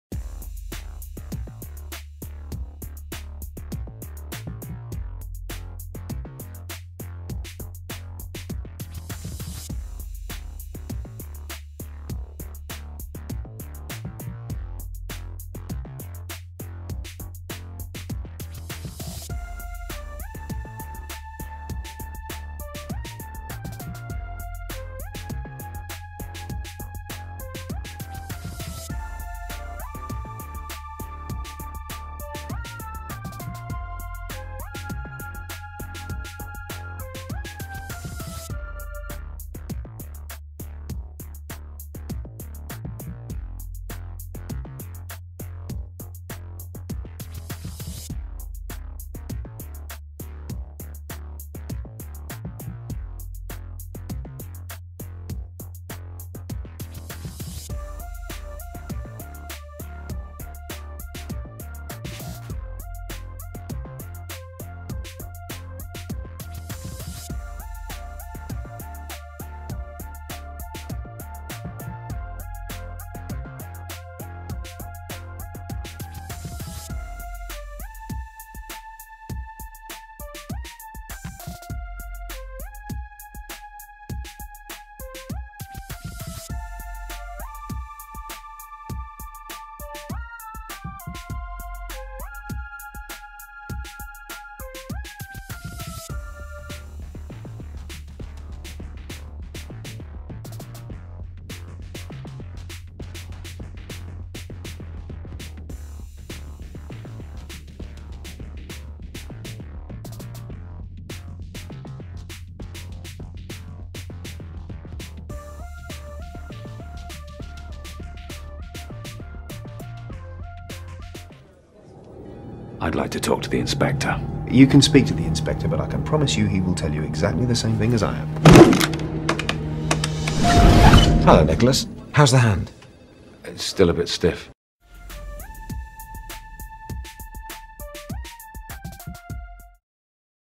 Edwin Lian Demoreel 2017
Here is my current demoreel that covers various aspect of my skills as a 3D generalist and as an animator.

You can look at my other demoreel that covers my animation under this link: